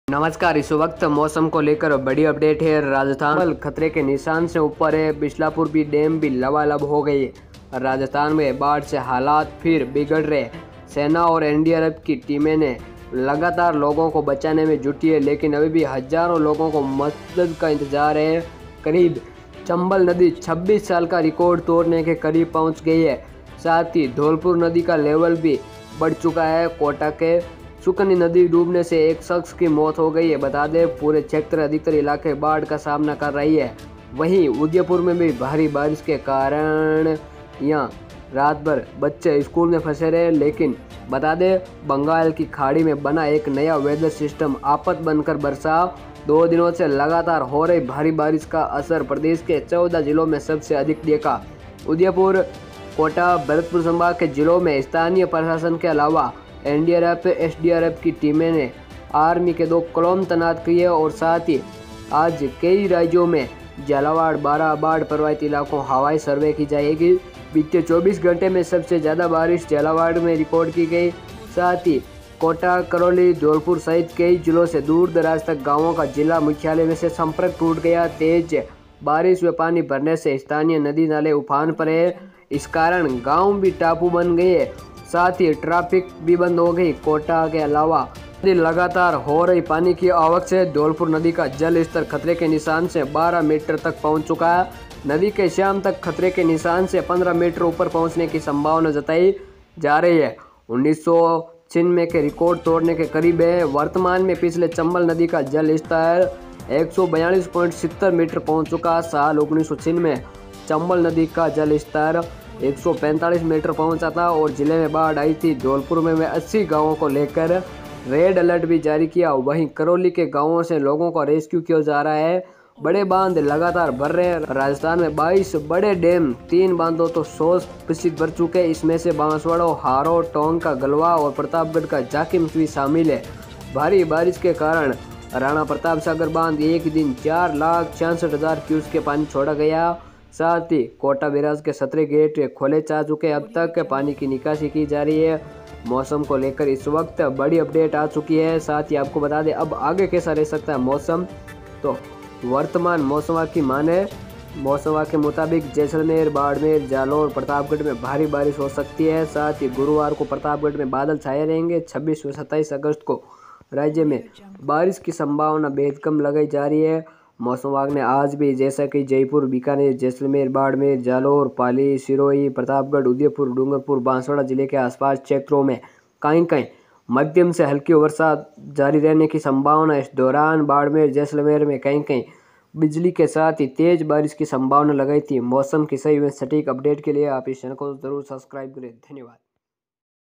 आज राजस्थान बाढ़ से डूब गया | कोटा बैराज के 17 गेट खुले , भारी बारिश होगी 24 घंटे | Rajasthan Mausam
Monsoon 2022 : रेगिस्तान कैसे बन गया समंदर ? कहां डूब गए बाढ़ से बचाने के वादे ? | Rajasthan

monsoon update Rajasthan: राजस्थान के हाड़ौती अंचल में भारी बारिश